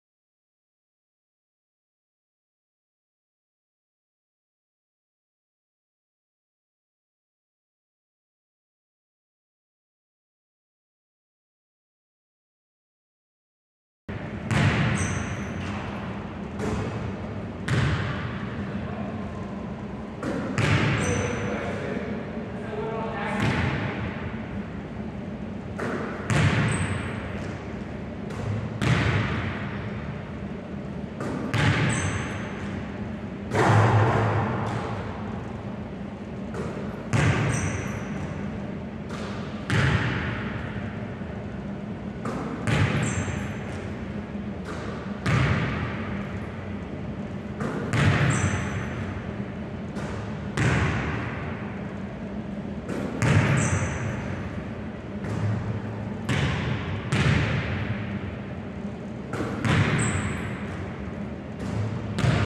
Nick Hadge/SD Mesa Coll: 3PT - Right Slot - 88% - 7/8 - 3/15/24
Nick Hadge
San Diego Mesa College
Guard
Freshman
5'11"
GPA - 3.94